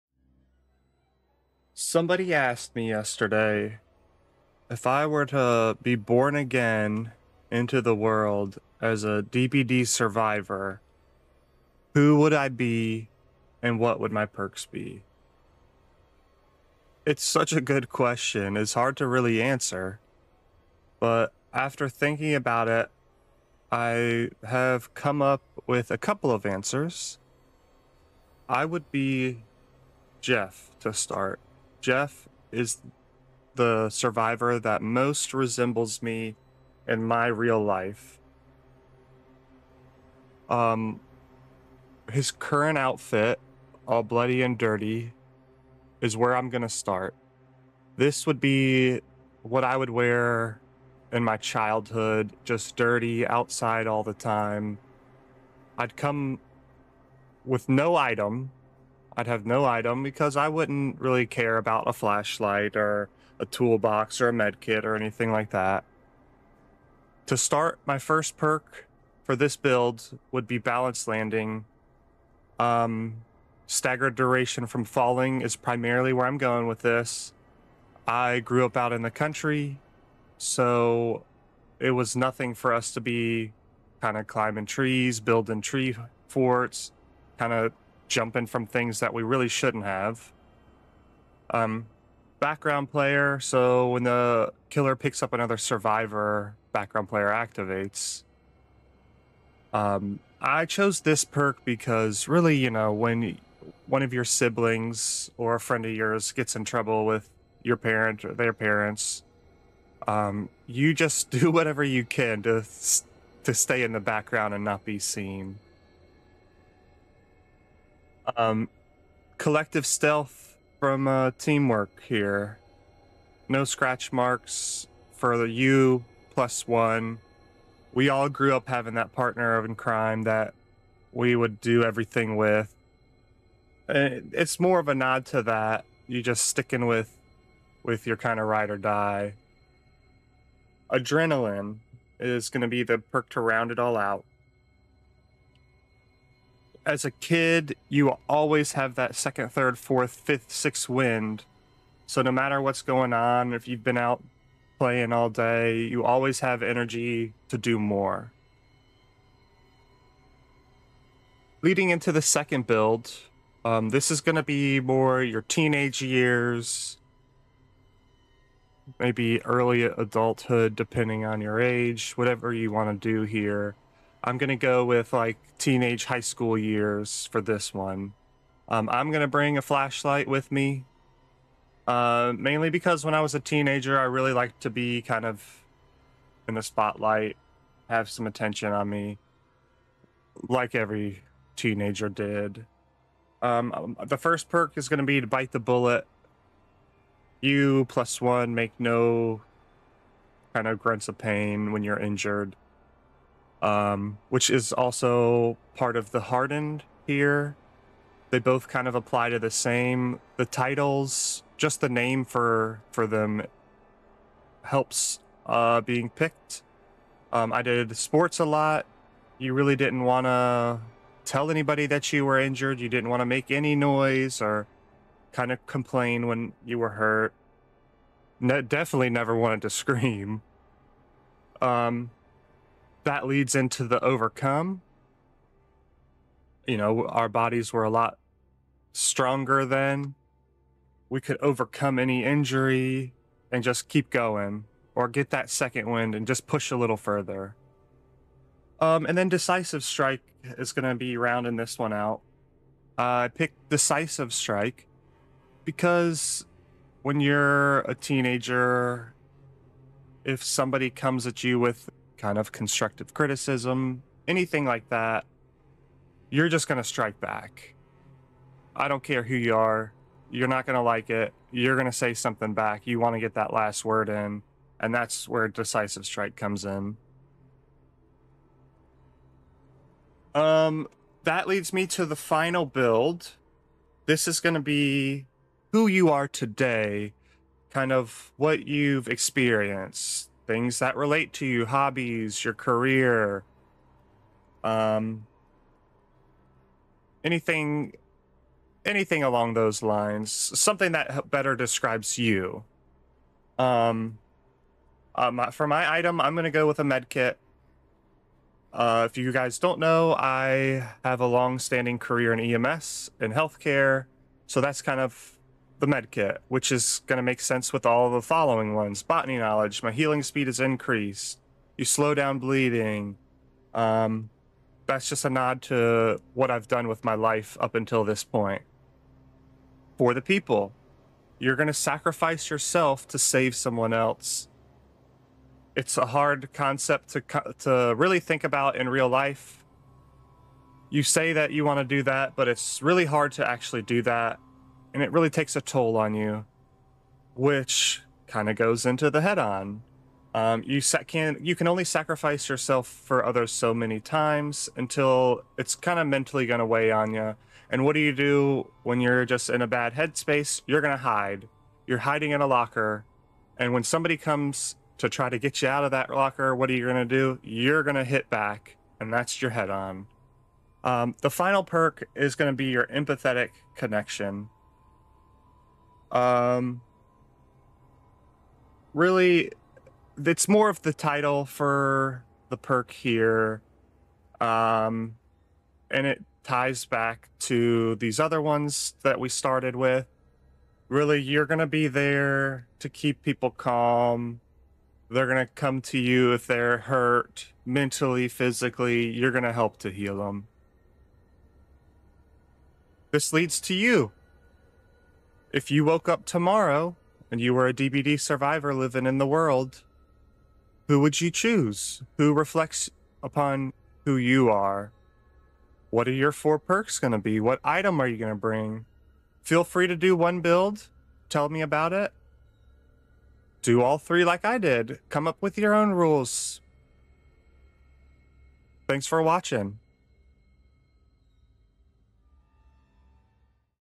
You wake up tomorrow a DBD survivor, who are you & what are your perks?
After being asked this same question on stream, I put thought into it, and here's my answer.  This is what I came up with!  I would love to hear what you came up with!